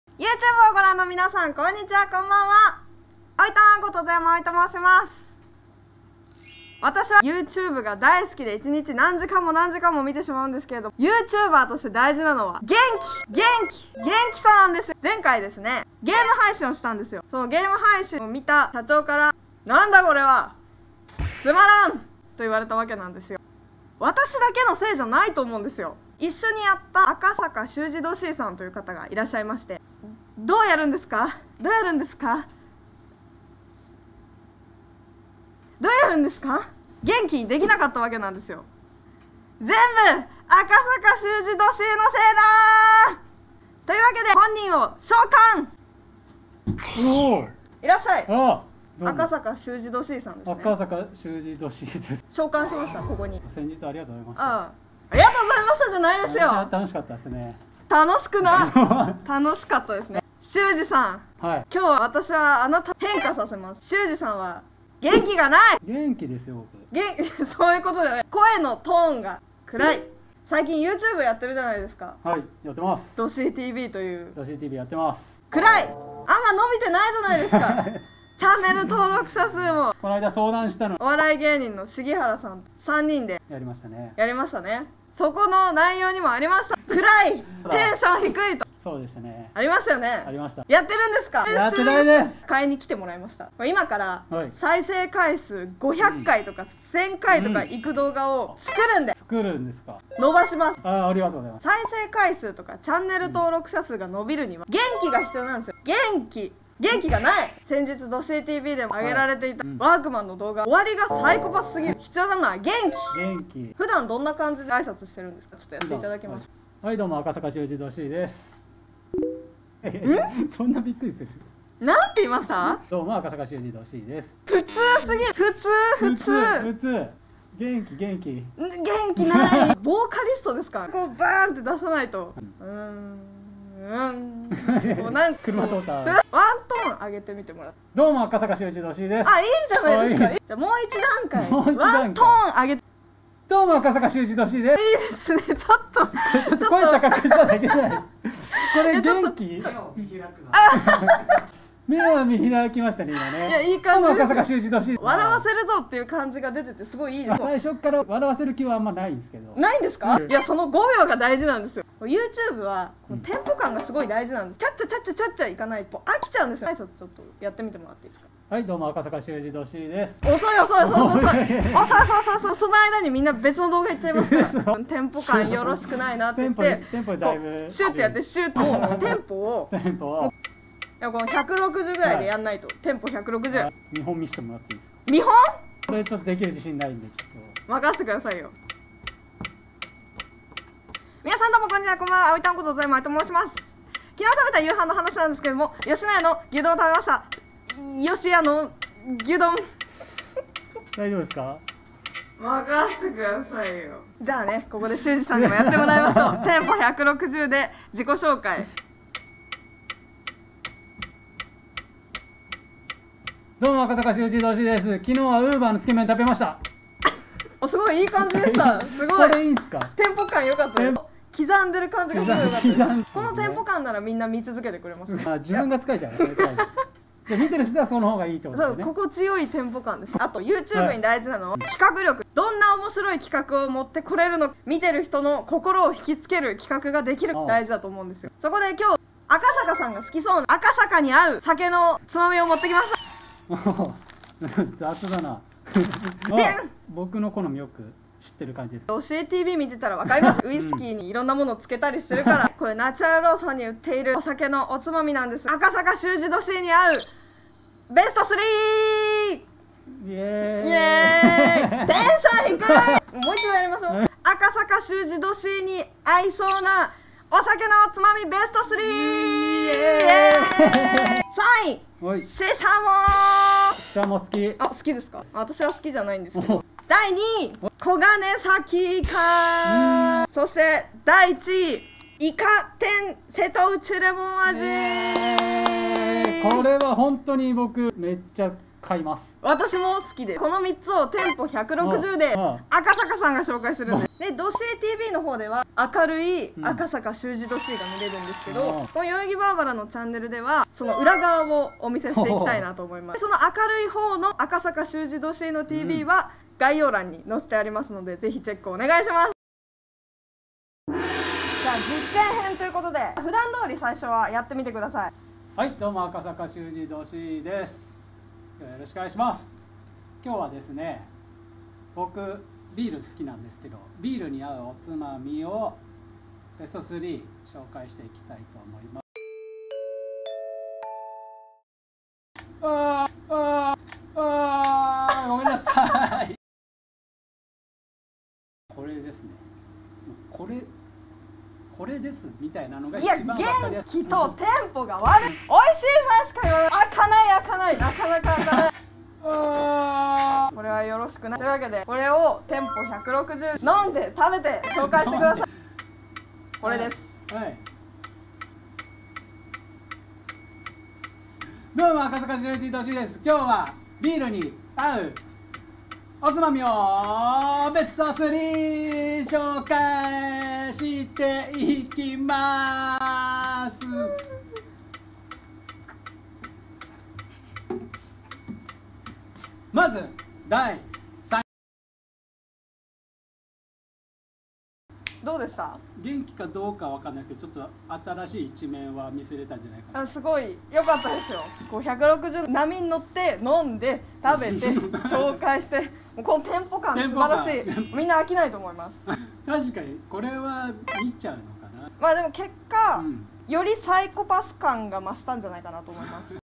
【とにかく明るい○○】テンポ160で酒のつまみベスト３を紹介してみた【赤坂シュウジ℃×とやまあおい】
赤坂シュウジ℃さんとのコラボ動画！

グッズ購入はこちら！！

〒151-0053　東京都渋谷区代々木1-42-4　代々木P1ビルB1